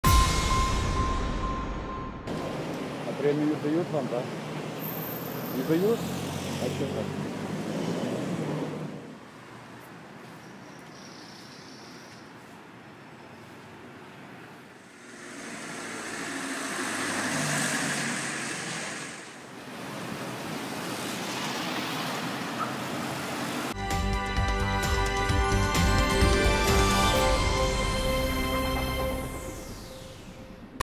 В Житомире на Большой Бердичевской у маршрутки на ходу отвалилось колесо - Житомир.info
24 сентября около 8 часов на ул. Б. Бердичевской, между улицами Гагарина и Довженко, у пригородной маршрутки №127 «Житомир- Пряжево» на ходу отвалилось заднее колесо.

В маршрутке находились пассажиры, никто серьезно не пострадал.